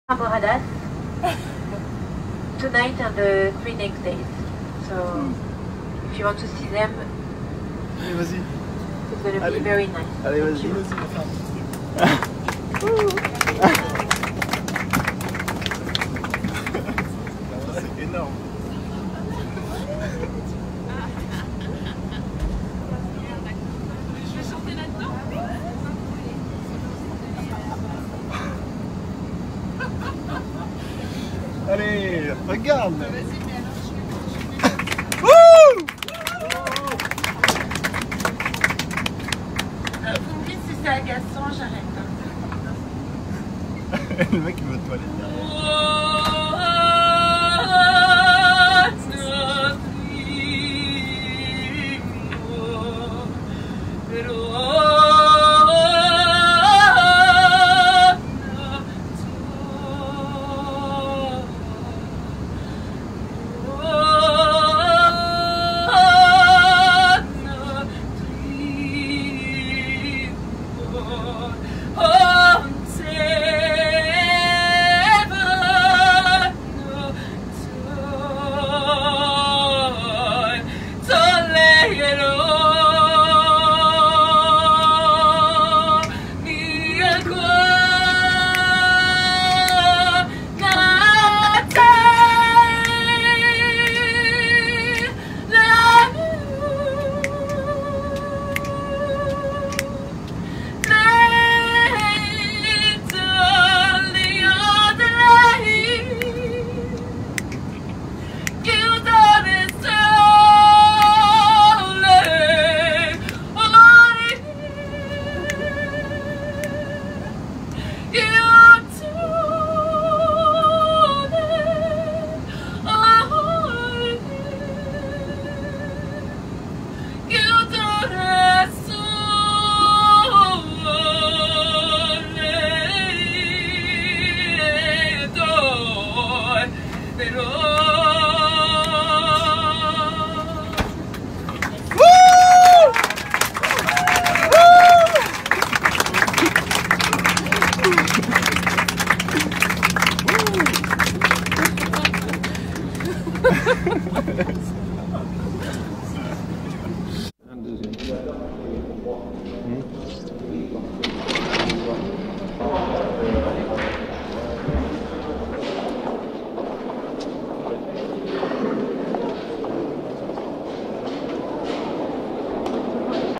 Laure Le Prunenec - Concert improvisé de Laure hier soir dans l’avion.
Message des demoiselles de l’air :
« Nous avons le groupe Rïcïnn dans notre avion, qui s’en va jouer au festival Extramuralhas de Leiria. La chanteuse va nous interpréter quelques chose. »
Admirez également le timing parfait de Mr-je-dois-absolument-aller-aux-toilettes-pendant-la-performance 😂😂